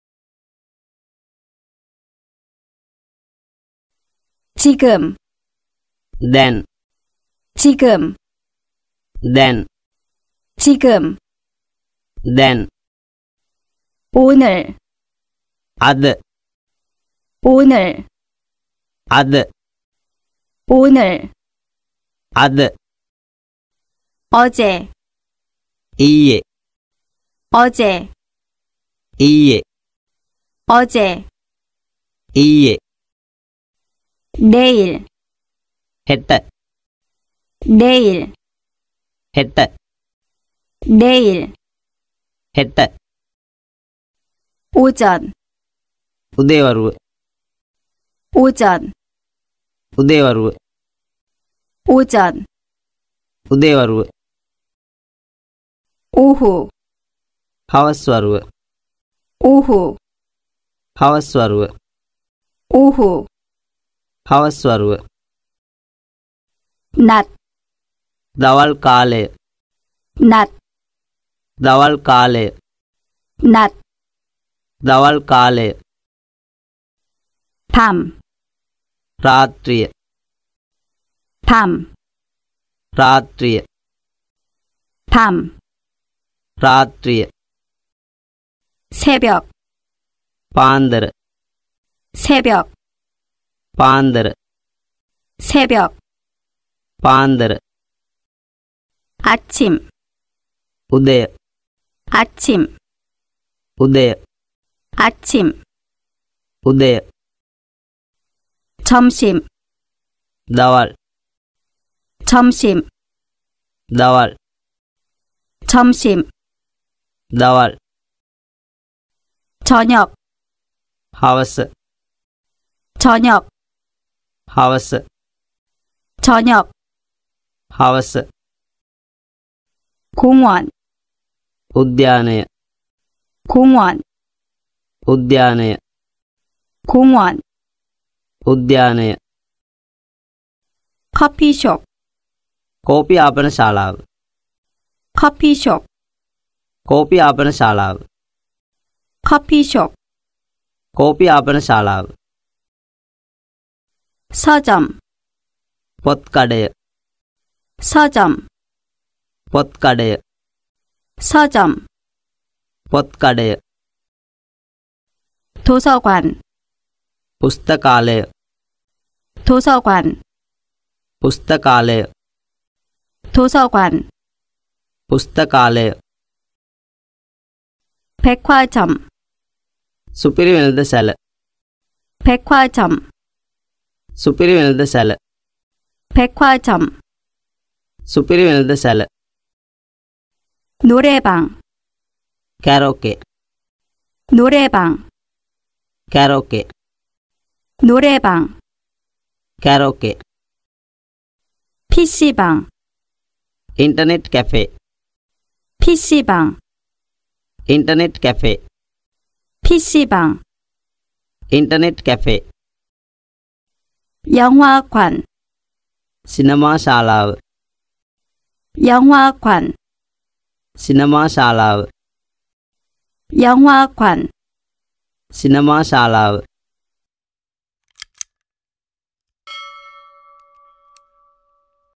Learn Korean in Sinhala | Korean Vocabulary part 03 | Korean words sinhala | koriyan wachana sinhala
Korean Vocabulary Part 03

in this video You can easily learn korean words by listening with pictures.
Ranga Institute Kurundugaha Hathekma